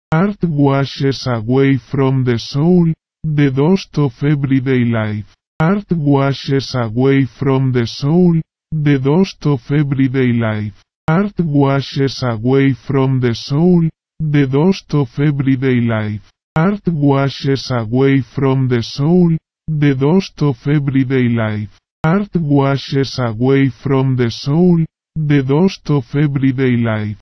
Picasso wise words
Picasso here saying " art washes away from the soul the dust of everyday
life "